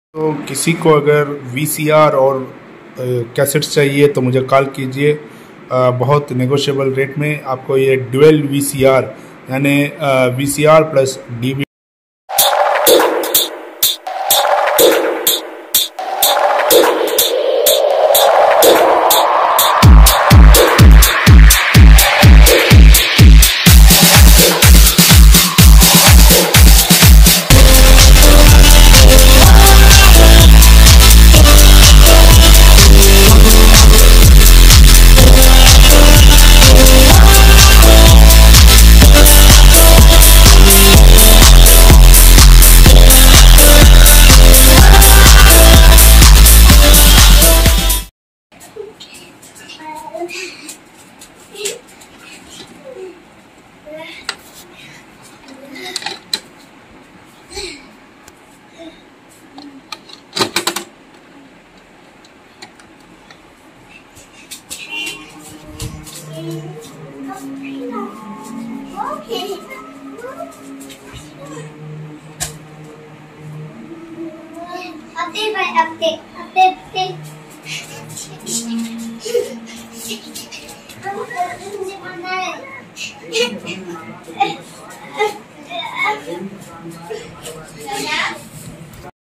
Dual VCR +=DVD Single piece for sale...Book now itself
Excellent condition Dual VCR and DVD Player for sale Single piece, what's up me for best price. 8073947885 Chaalbaaz VHS cassette free with this set No remote